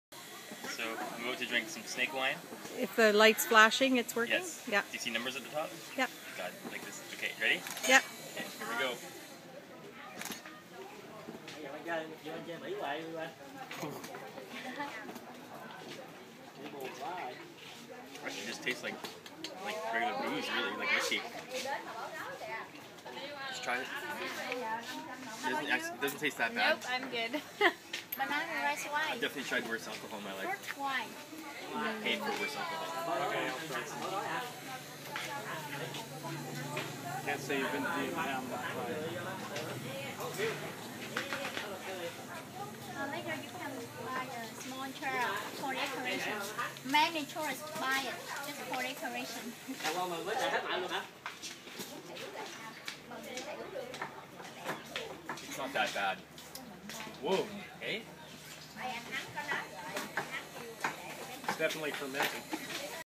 Shots of Snake Wine
In some little village hideaway in the Mekong Delta, snakes are a big part of the lifestyle.  They make snake wine, and I do a shot!